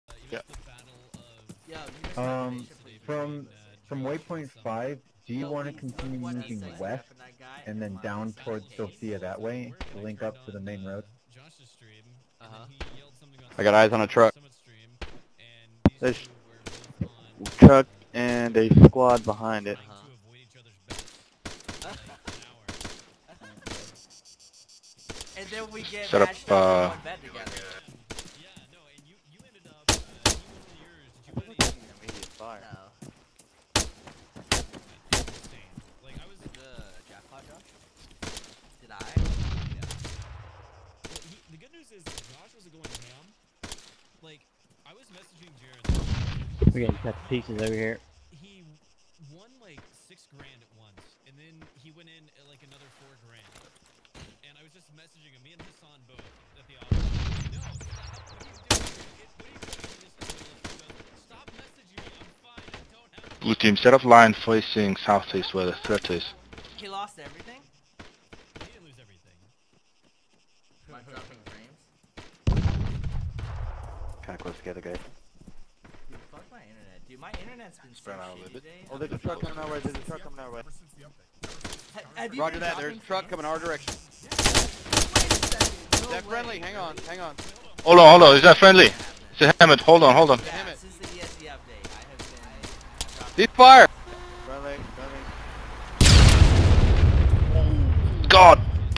no hope @ TacticalGamer
TacticalGamer.net

10+ guys all low on ammo, fought through 40+ EI, Tanks, snipers, and static MGs... finally have an extraction vehicle and the EI allow it within 10ft of us before blowing it up.